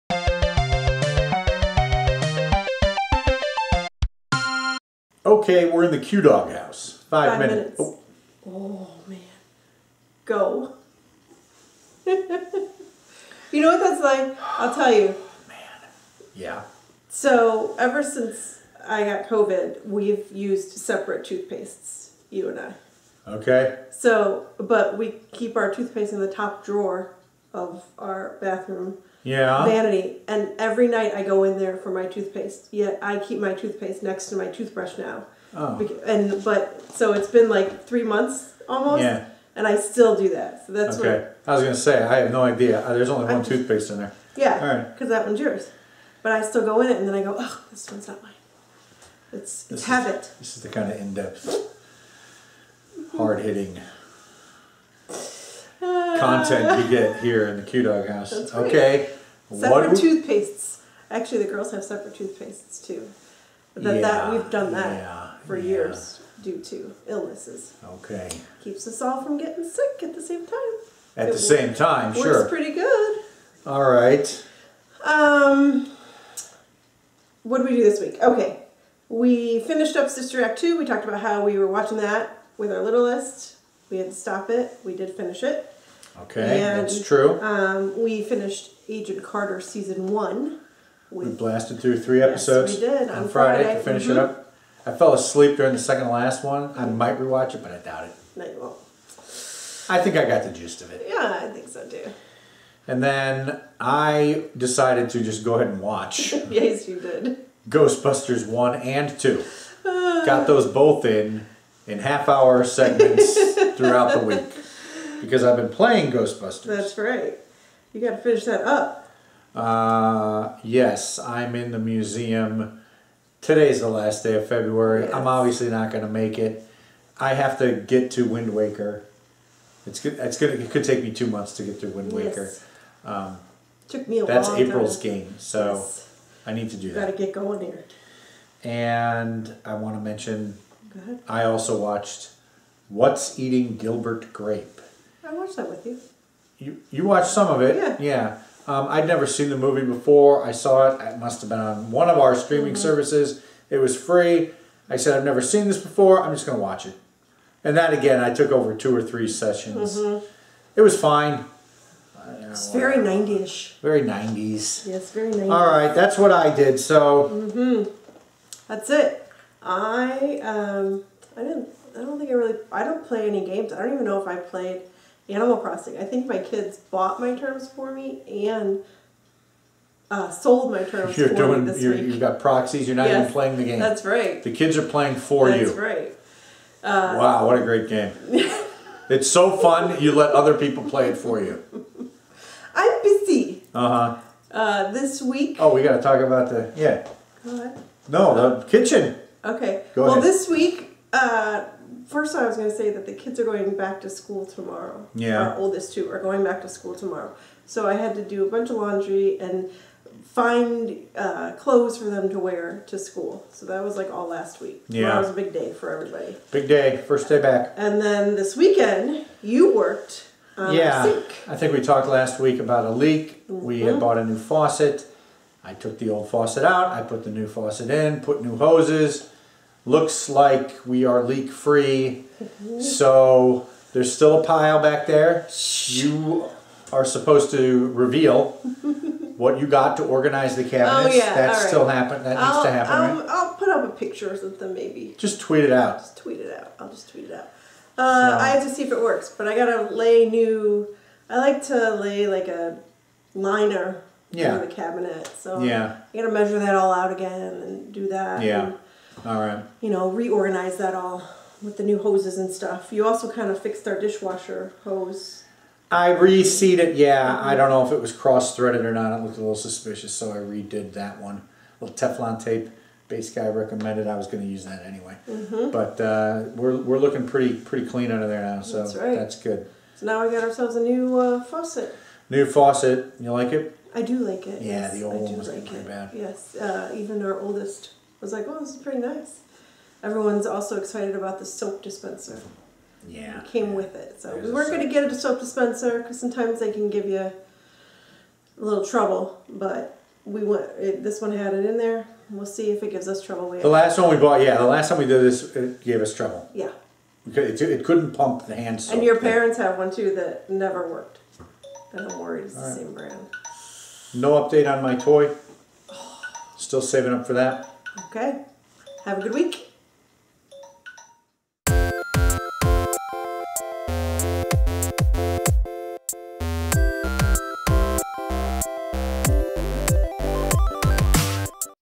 In the QDawg House (March 2, 2021)
Welcome to our "In the QDawg House" vlog for March 2, 2021. Toothpaste habits. Agent Carter Season 1 is done. Eric is playing Ghostbusters and decided to re-watch Ghostbusters 1 and 2. Eric finally watches "What's Eating Gilbert Grape". New faucet installation. Check out the video and let us know about your week!